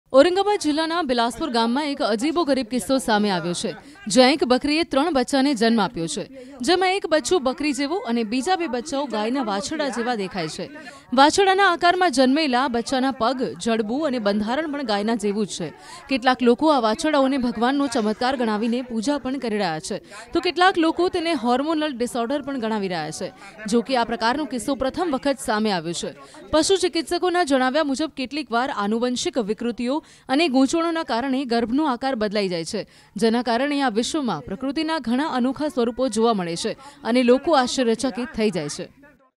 Aurangabad મા બકરીએ 2 વાછરડાને જન્મ આપ્યો ! Viral Video | Uttar Pradesh | Goat | Animal Video | N18V
A strange case has come to light in Bilaspur village of Aurangabad district... where a goat gave birth to three kids..., one kid looks like a goat and two look like cow calves. The kids born in the shape of calves have legs, jaw and The constitution is also similar to that of a cow.

ઔરંગાબાદ જિલ્લાના બિલાસપુર ગામમાં એક અજીબોગરીબ કિસ્સો સામે આવ્યો છે... જ્યાં એક બકરીએ ત્રણ બચ્ચાને જન્મ આપ્યો છે..., જેમાં એક બચ્ચું બકરી જેવો અને બે બચ્ચાઓ ગાયના વાછરડા જેવા દેખાય છે. વાછરડાના આકારમાં જન્મેલા બચ્ચાના પગ, જડબું અને બંધારણ પણ ગાયના જેવું જ છે. કેટલાક લોકો આ વાછરડાઓને ભગવાનનો ચમત્કાર ગણાવીને પૂજા કરી રહ્યા છે તો કેટલાક તેને હોર્મોનલ ડિસઓર્ડર ગણાવી રહ્યા છે...